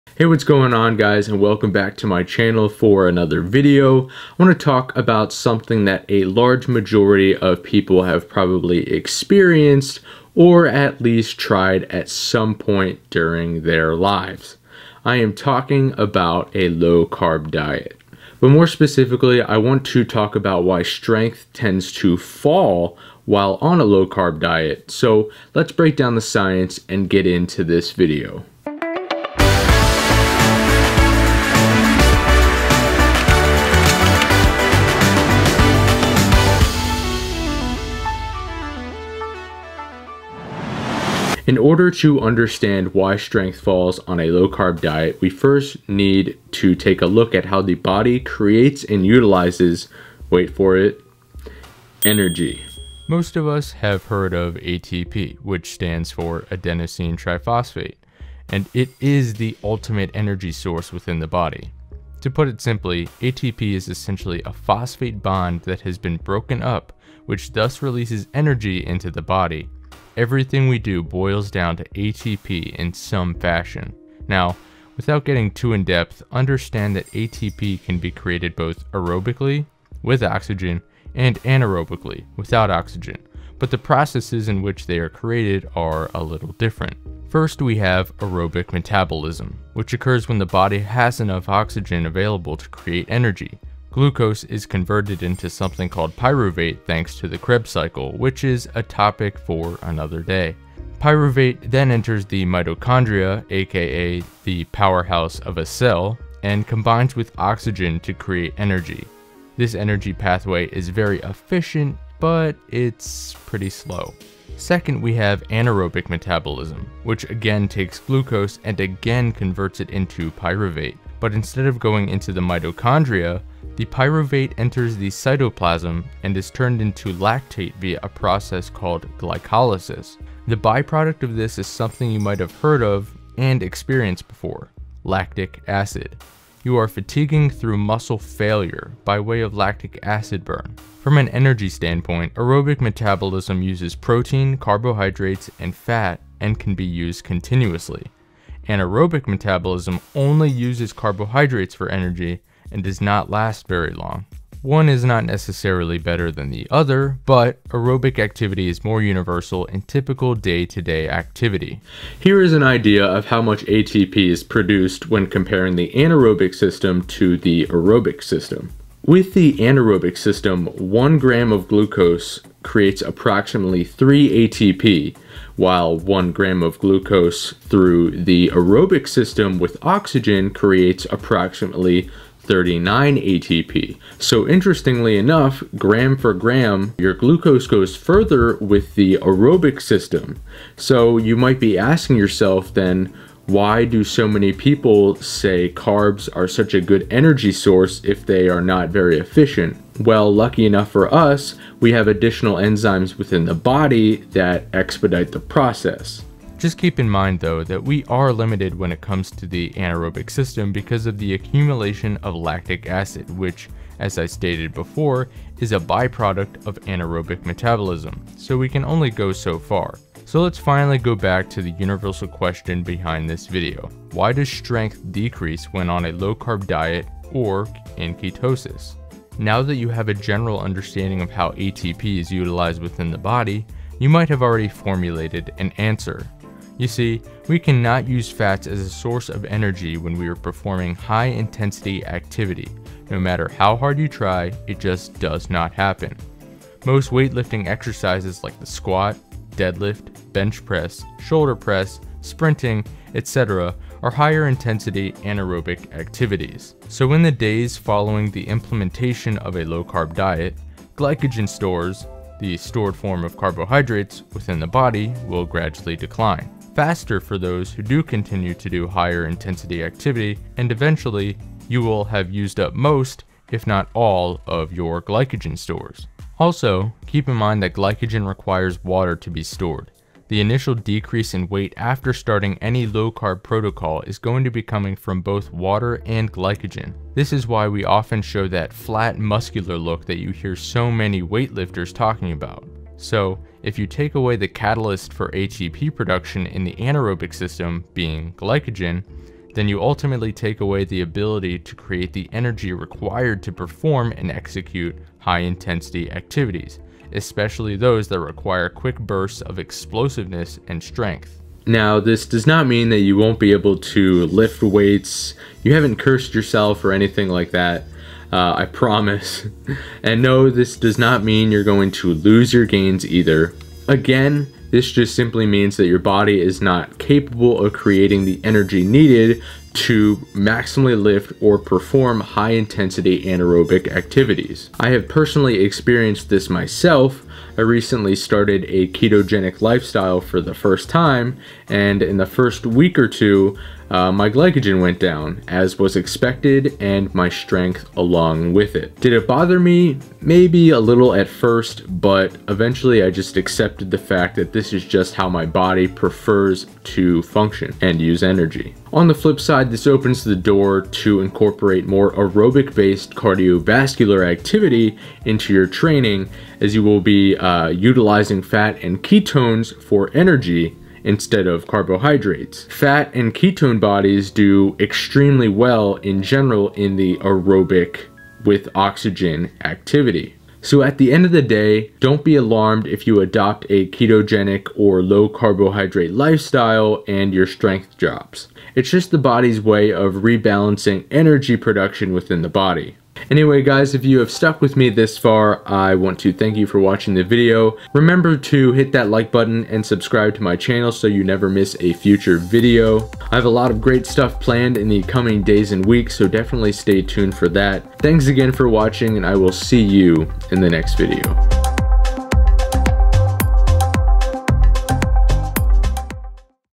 Why Does Strength Decrease On A Low Carb & Ketogenic Diet?
► Personalized ONLINE COACHING!

In order to understand why strength decreases on a low carb
or ketogenic diet, we first need to take a look at how the body creates and utilizes energy.

Most of us have heard of ATP, which stands for Adenosine Triphosphate, and is the ultimate energy source within the body. To put it simply, ATP is essentially a phosphate bond that has been broken up which thus releases energy into the body. Everything we do boils down to ATP in some fashion.

Now, without getting too in depth, understand that ATP can be created both aerobically (with oxygen) and anaerobically (without oxygen), but the processes in which they are created are a little different.

First, we have Aerobic Metabolism - which occurs when the body has enough oxygen available to create energy. Glucose is converted into something called Pyruvate thanks to the Kreb Cycle, which is a topic for another day… Pyruvate then enters the mitochondria, a.k.a. “The Powerhouse of A Cell”, and combines with oxygen to create energy. This energy pathway is very efficient, but is slow...

Second, we have Anaerobic Metabolism - which again takes glucose and again converts it into Pyruvate. But instead of going into the mitochondria, the pyruvate enters the Cytoplasm and is turned into lactate via a process called Glycolysis. The byproduct of this is something you might have heard of before - lactic acid. You are fatiguing through muscle failure by way of lactic acid burn.

From an energy standpoint, Aerobic metabolism uses protein, carbohydrates, and fat, and can be used continuously. Anaerobic metabolism only uses carbohydrates and does not last very long. One is not necessarily better than the other, but aerobic activity is more universal in typical day-to-day activity.

Here is an idea of how much ATP is produced when comparing the Anaerobic System to the Aerobic System:

With the anaerobic system, 1g of glucose creates approximately 3 ATP while 1g of glucose through the aerobic system with oxygen creates approximately 39 ATP.

So interestingly, gram for gram, your glucose goes further with aerobic metabolism. So you might be asking yourself why so many people say carbs are good for energy if they are so efficient? Well, lucky enough for us, we have additional enzymes within the body that help expedite the process.

Just keep in mind though, that we are limited when it comes to the anaerobic system because of the accumulation of lactic acid, which as I stated before is a byproduct of anaerobic metabolism, so we can only go so far.

So let’s finally go back to the universal question behind this video - why does strength decrease when on a low carb diet or Ketosis?

Now that you have an understanding of how ATP is utilized within the body, you might have already formulated an answer...

You see, we cannot use fats when we are performing high-intensity activity - no matter how hard you try, it just does not happen. Most weightlifting activities like the squat, deadlift, bench press, shoulder press, sprinting, etc, are higher intensity, anaerobic activities.

So, in the days following a low carb diet, glycogen stores (the stored form of carbs) within the body will gradually decline - faster for those who do continue to do higher intensity activity - and eventually, you will have used up most, if not all of your glycogen stores.

Also, keep in mind that glycogen requires water to be stored. The initial decrease in weight after starting any low carb protocol is going to be coming from both water and glycogen. This is why we often show that “flat” muscular look that you hear so many people talking about.

So, if you take away the catalyst for ATP production in the Anaerobic System - being glycogen, then you take away the ability to create the energy required to perform and execute high-intensity activities.

Now, this does not mean you won’t be able to lift weights, you haven’t cursed yourself or something like that… And no, this does not mean you are going to lose your gains either. This just simply means that your body is not capable of creating the energy needed to maximally lift or perform for high intensity, anaerobic activities.

On the flip side, this opens the door to incorporate more aerobic-based cardiovascular activity into your training as you will be utilizing fat and ketone bodies for energy, which does extremely well in aerobic (oxygen) activity.

So at the end of the day, don’t be alarmed if you adopt a low carb or ketogenic lifestyle and your strength drops. It’s just the body’s way of rebalancing energy production within the body.